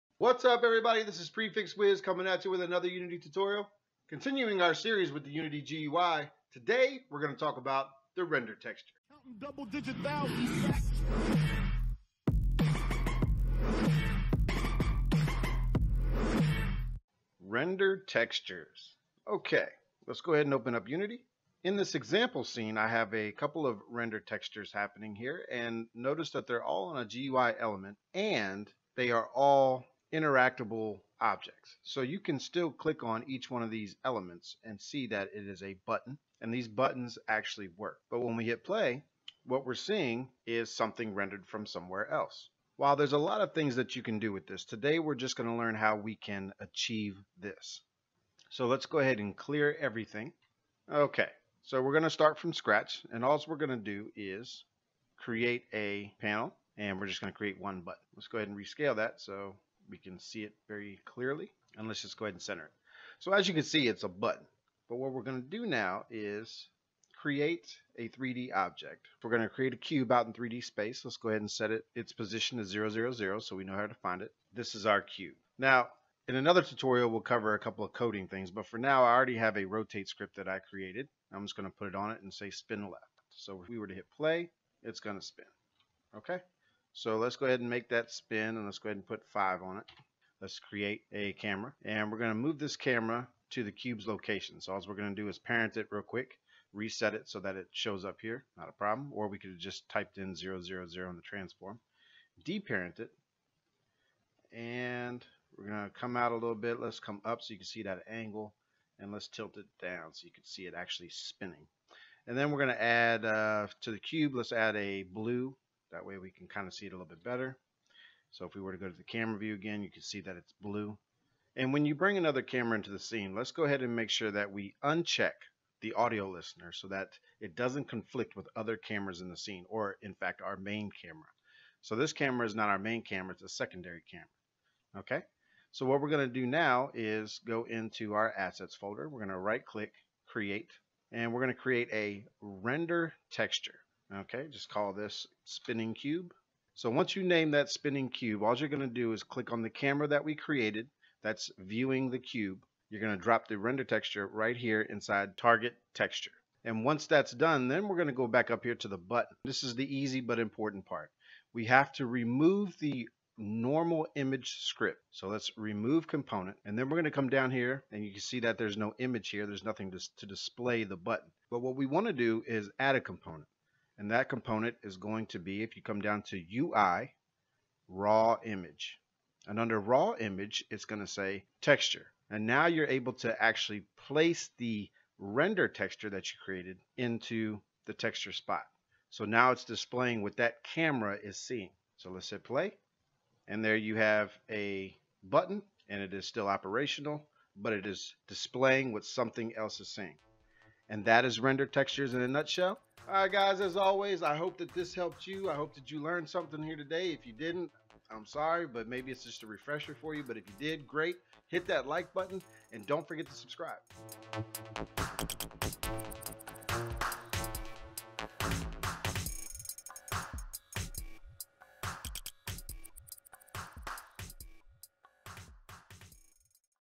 Unity GUI: Unity GUI Render Texture 2017.2
In this Unity Tutorial, I will show you how to create and use a Unity GUI Render Texture to help expand your projects creativity!